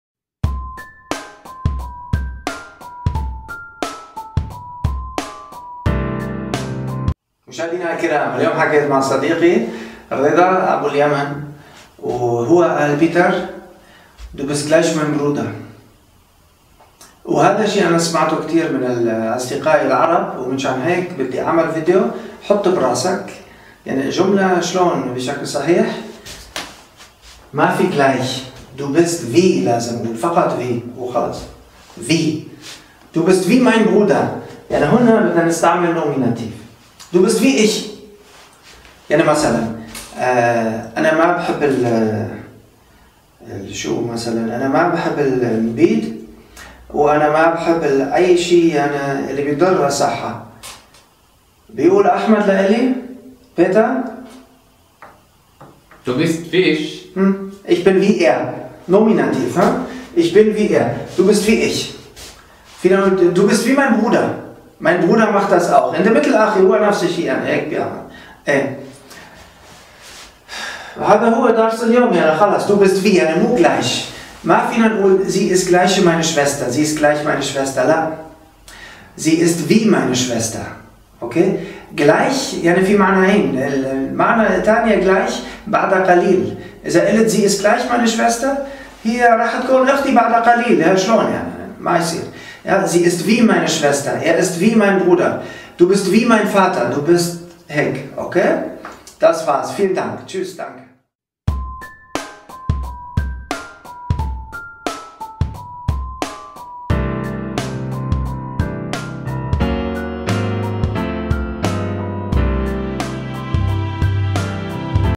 حطه برأسك - أنت مثل ... HÄMMER'S DIR REIN - DU BIST WIE
في هذا الفيديو نتعلم كيف منقول "أنت مثلي" في الألمانيا.

In diesem Video lernst du, wie du auf Deutsch sagen kannst: Du bist wie ich, wie mein Bruder ...